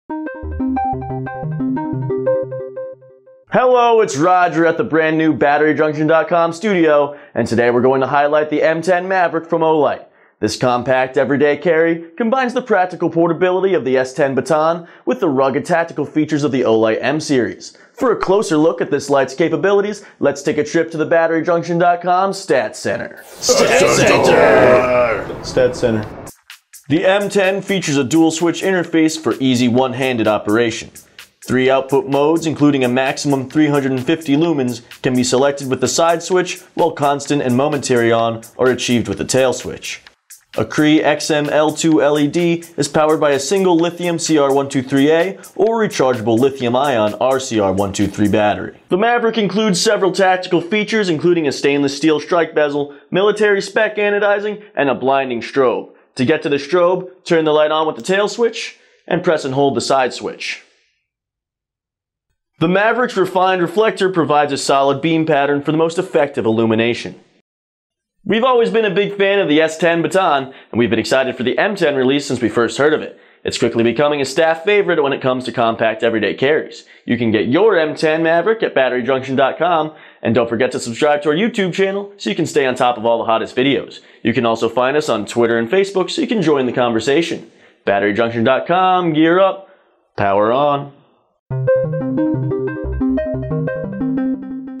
Olight M10 Maverick - Tabletop Overview From Battery Junction
The Olight M10 Maverick continues the world-famous M series of tactical lights by building off of one of Olight's most popular lights. The 1 x CR123A side switch S10 platform has been modified with a tactical tail switch and crenelated stainless steel bezel to make this compact and tactically sound device. Capable of producing up to 350 lumens from the advance XM-L2 LED, the M10 generates a more powerful beam than lights several times its size. A removable pocket clip allows the M10 to be used as a practical Every Day Carry while a variety of weapon mounts and a blinding strobe feature maintain its versatility as a duty light. Tough scratch resistant anodizing protects the aluminum body from scratches and corrosion. Three outputs and the strobe function are accessed by pressing the side switch while momentary/constant on are achieved with the tail switch.

Features:
Dual switches: Push-button tail cap switch with momentary-on function; while side switch to change the output
Intelligent driver circuit with memory function, automatically memorize the output level before shutdown (exclude strobe)
Thick stainless steel bezel protects the lens
Special designed deep reflector, for a perfect focused beam
Aviation grade aluminum body, with anti-scratch type III hard anodizing
Lens: Toughened ultra-clear glass with anti-reflective coating
Slip-resistant body design
With a two-direction invertable pocket clip, easy and reliable for use
Reverse polarity protection guards against improper battery installation

Specifications:
LED: Cree XM-L2
Runs on: 1 x CR123A OR 1 x RCR123 battery
Output/Runtime:
High: 350 Lumens/1.3 Hours
Medium: 80 Lumens/7.5 Hours
Low: 5 Lumens/108 Hours
Peak beam intensity: 2880cd
Max beam distance: 107 meters
Dimensions:
Length: 3.66" (93mm)
Diameter: 0.9" (23mm)